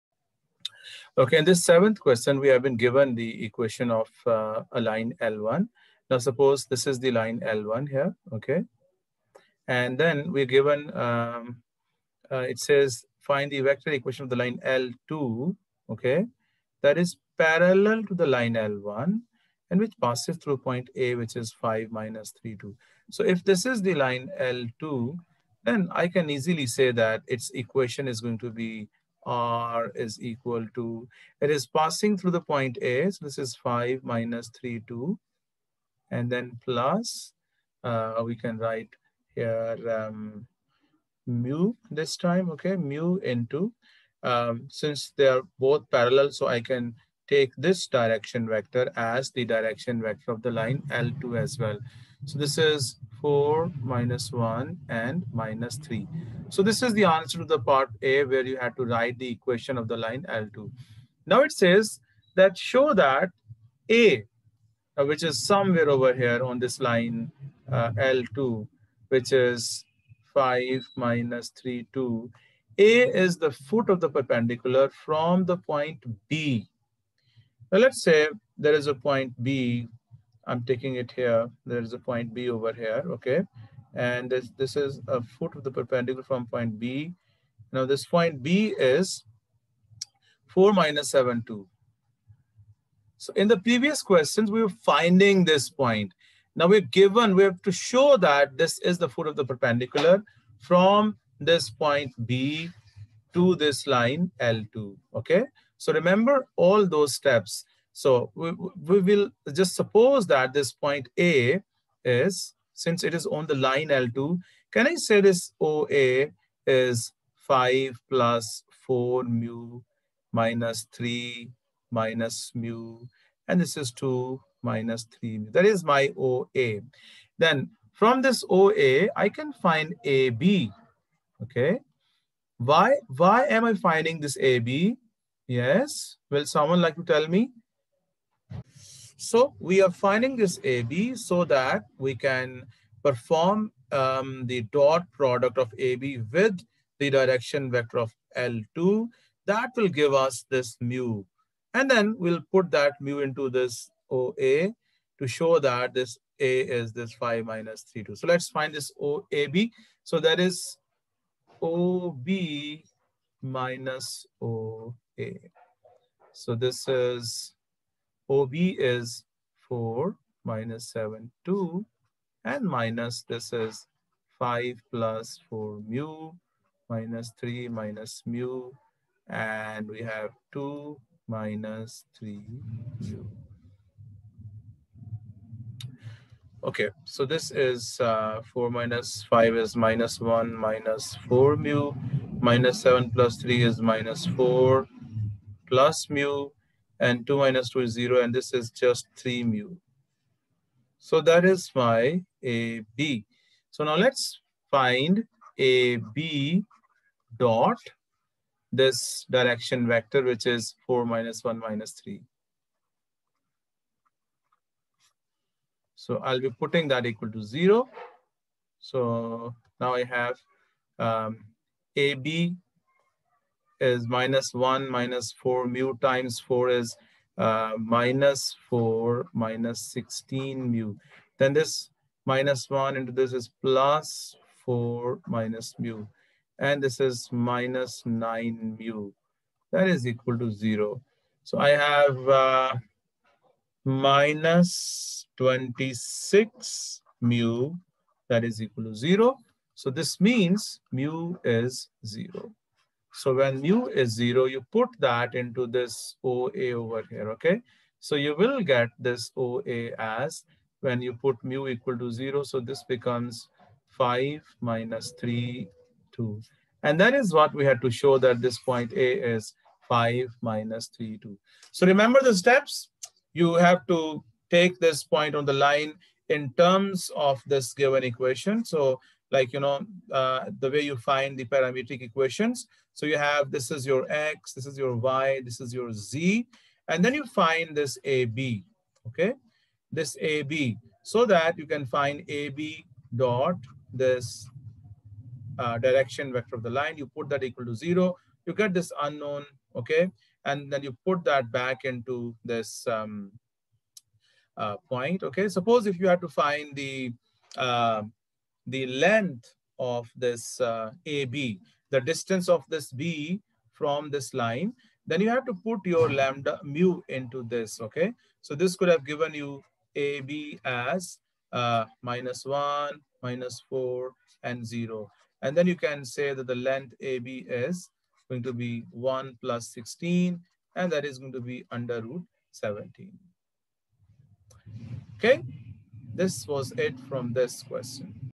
How to Find the Foot of the Perpendicular from a Point onto a Line. Pure Maths 3/Vectors.
How to find the position vector of a point that is the foot of the perpendicular from a given point to a line whose vector equation is given.
EX 9 D, Question 7/ P 3 Vectors/ A Level  Maths.